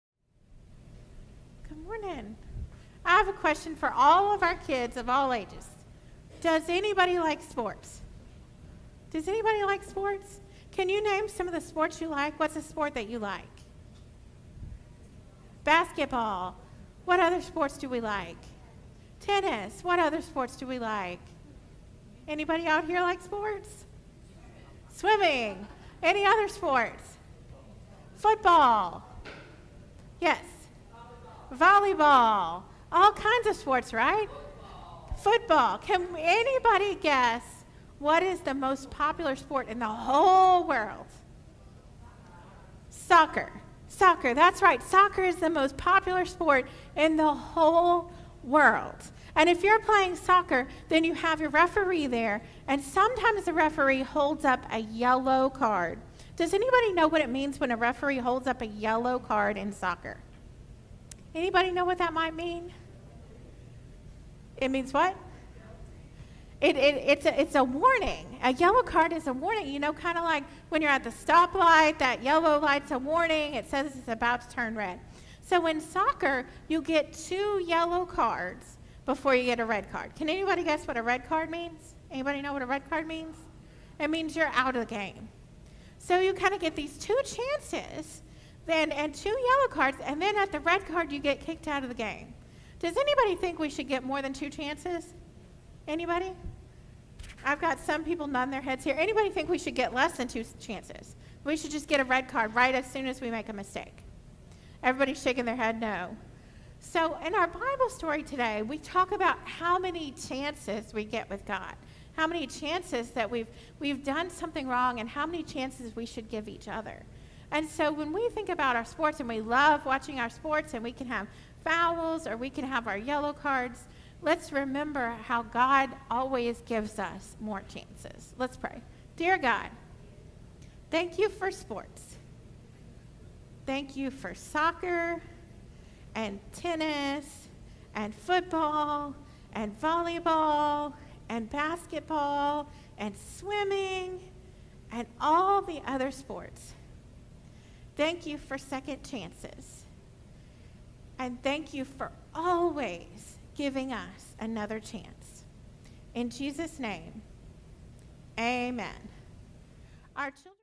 How Many Chances? - Children's Sermon
Recorded September 17, 2023 at Algiers United Methodist Church in New Orleans, Louisiana. Rev. JoAnne Pounds